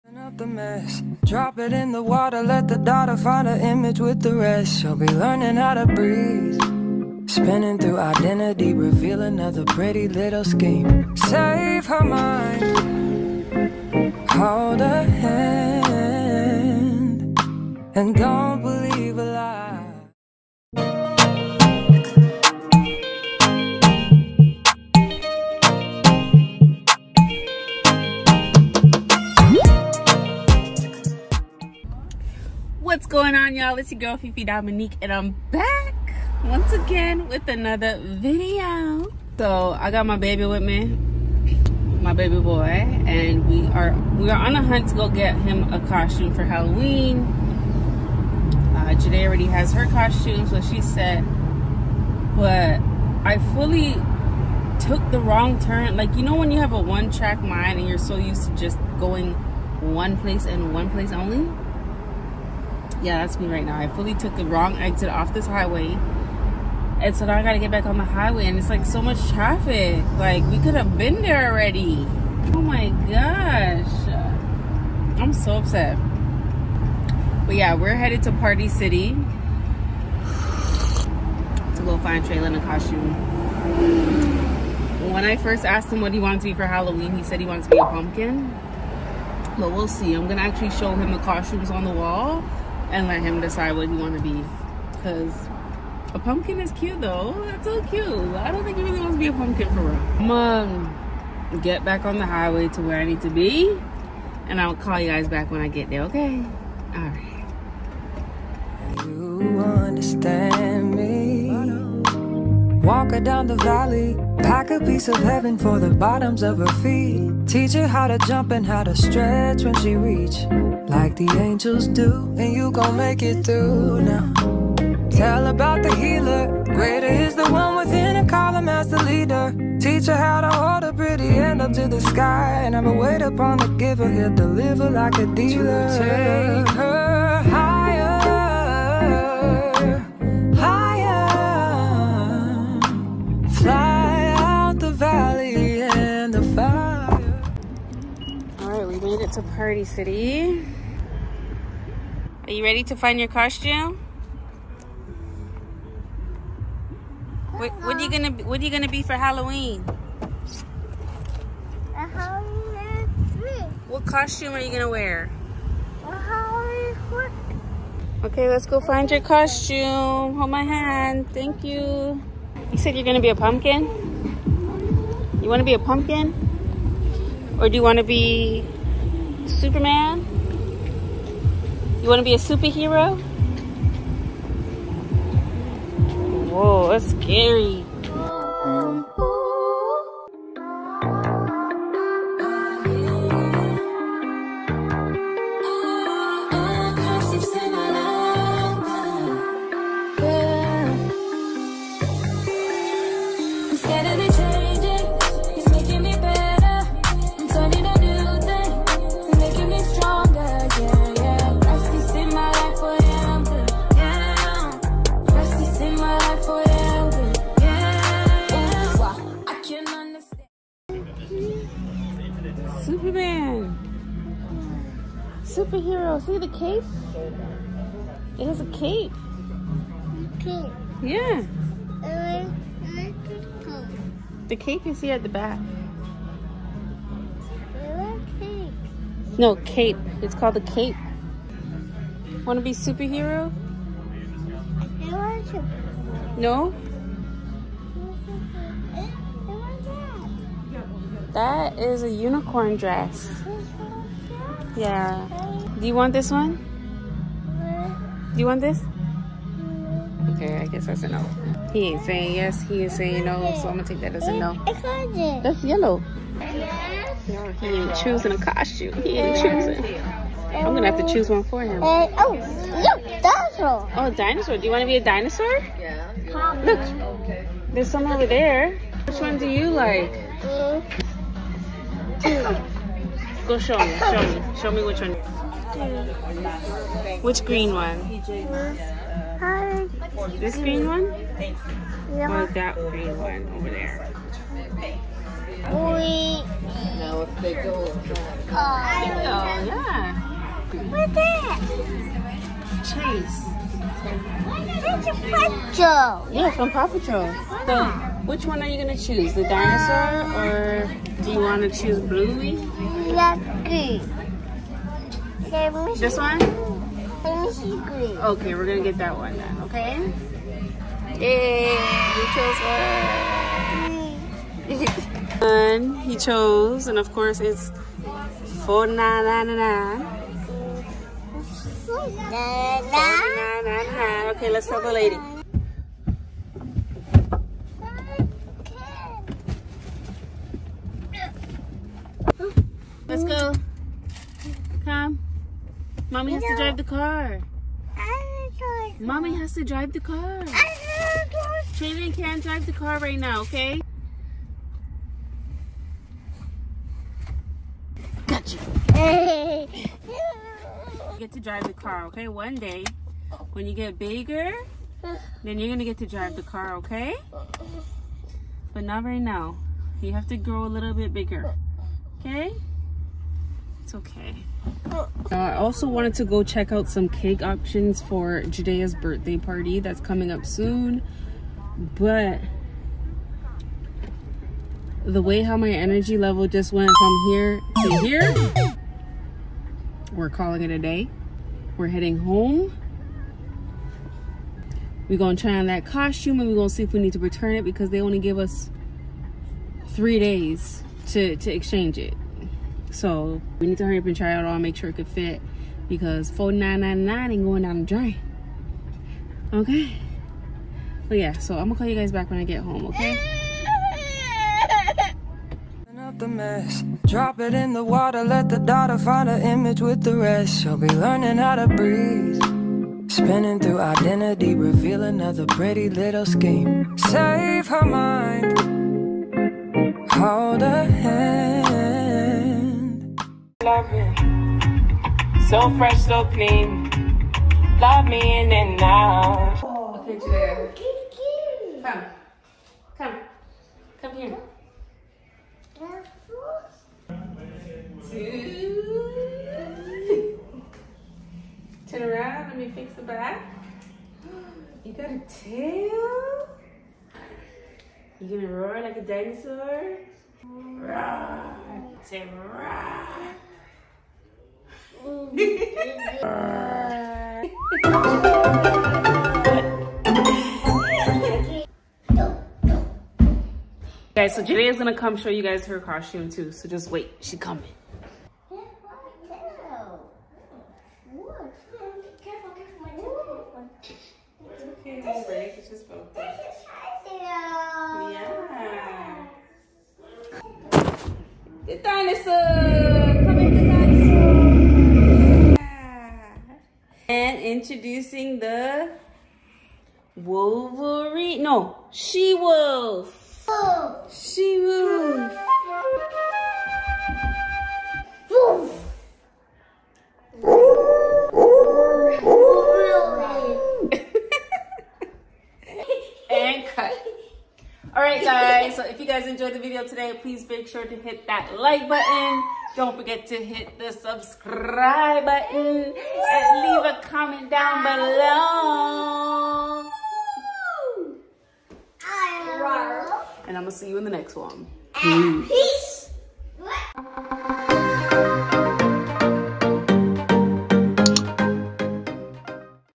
MOM VLOG: Halloween Costume Shopping with a Toddler | Party City Chronicles 🎃
In todays video I took my toddler to party to find a Halloween costume. It was such a struggle because he was having a hard time choosing which costume he wanted. 1/10 wouldn’t recommend taking your toddler to party to try to find a costume. It was just waaay too many options for him to choose from lol. Anyway, he finally made a decision after a good 45 mins! Princess Jadaiah also made a debut and showed off her costume in the end. My heart is full because they were so happy!